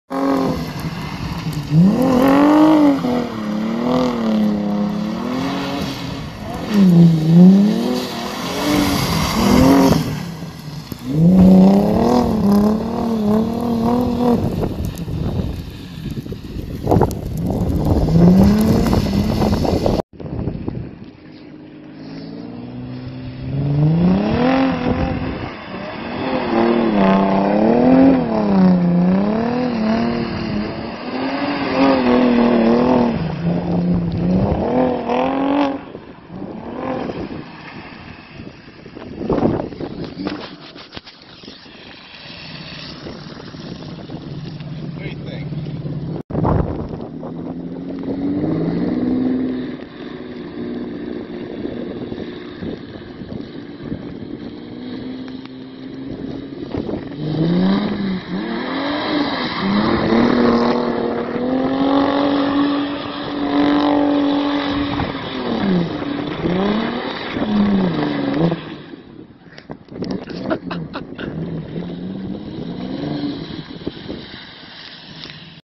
Playing with the stage 2 B5 S4 in some very light snow
The little bit of snow we had I needed to get  a dount and drift or two at least :-) Giac stage 2  B5 S4, 3 inch downpipes, 2.5 inch exhaust out the back , jhm racing transmission with rear wheel bias, lightweight flywheel with but RS4 pressure plate,aem dryflo intake with carbon fiber 034 cold air box,apr bi pipe, Bilstein big bore shocks with Tein lowering springs, jhm stabilizer bar and 3rd trans mount, other here and there's I'm not going to list it all off